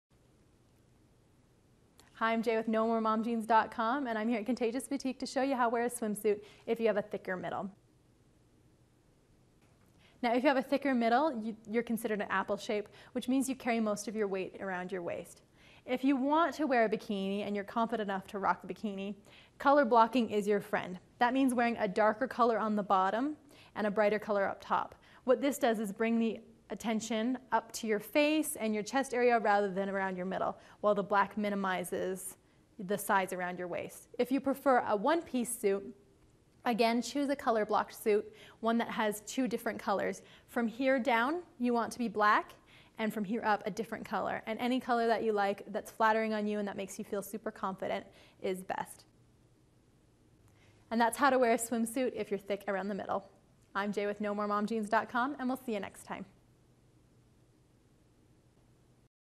What Kind of Bathing Suit Is Good for a Thick Waist?
What Kind of Bathing Suit Is Good for a Thick Waist?. Part of the series: Bathing Suits. Different types of bathing suits are designed for specific body types. Find out what kind of bathing suit is good for a thick waist with help from an experienced health, fitness, parenting and lifestyle writer in this free video clip.